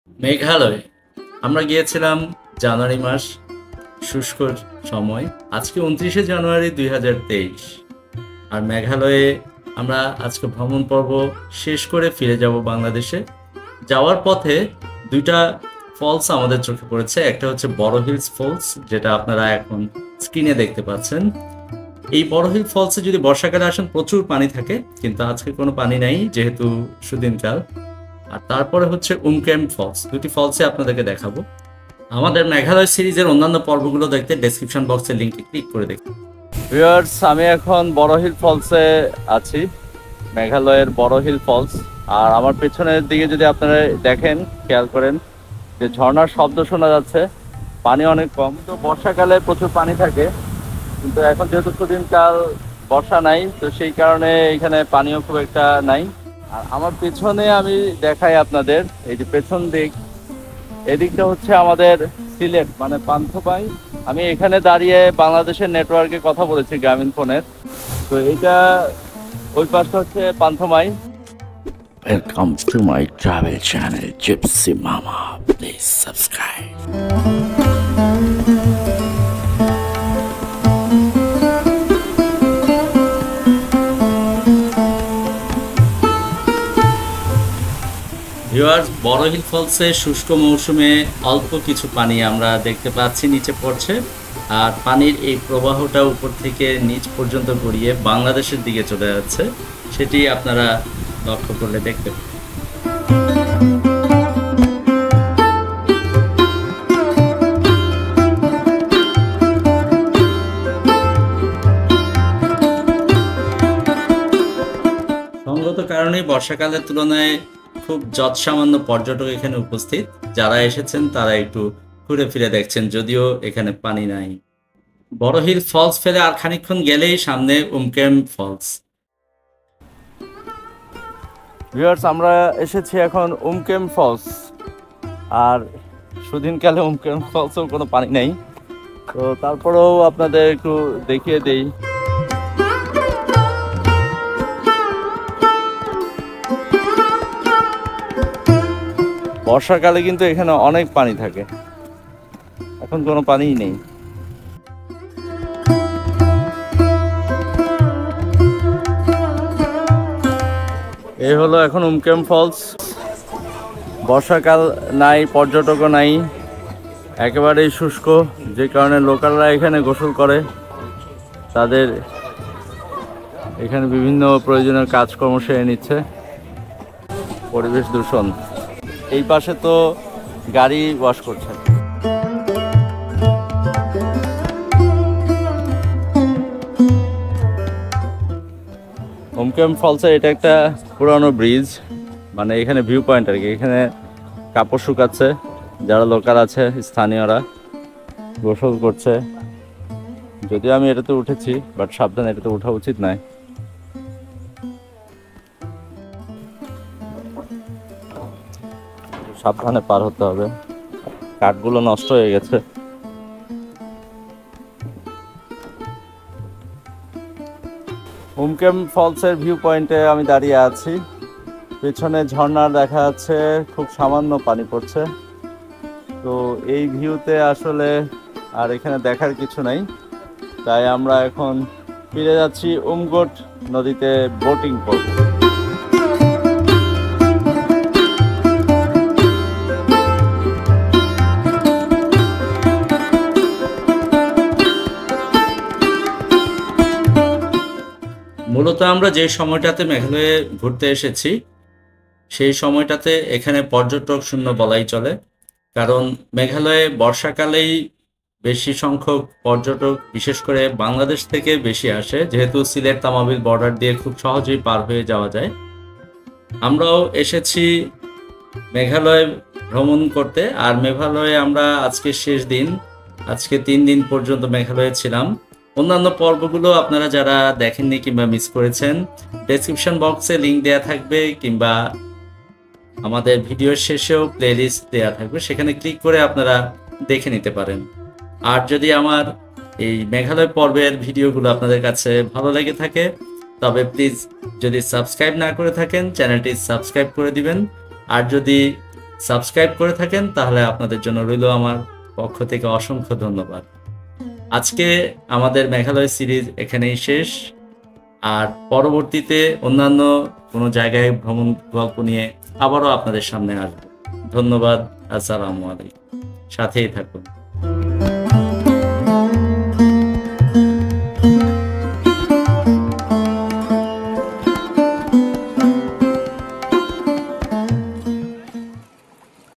পানিহীন ওয়াটার ফলস মেঘালয়ে ।। Boro Hills Falls and Umkrem Falls, Meghalaya@Gypsy Mama
Boro Hills and Umkrem Falls ,Meghalaya
আমাদের মেঘালয় সিরিজের আজকের শেষ পর্বের ভিডিও এই বড় হিলস ও উমক্রেম ফলস যা বাংলাদেশের সিলেট জেলার পাশেই। পান্থমাই ঝর্না নামে আমরা সিলেটে অনেকে জানি। আমাদের ভিজিটের সময় শুস্ক মৌসুম বিধায় কোন পানি ছিল না ঝর্নায়। আমরা তিন দিনের মেঘালয় ট্যুর করে আজ ফিরে যাচ্ছি বাংলাদেশ এ তাই যাবার পথে আমাদের দেশের পর্যটকদের সুপরিচিত এই দুইটি ওয়াটার ফলস দেখালাম আপনাদের। সব পর্ব দেখতে নিচের লিঙ্কে ক্লিক করুন প্লিজ।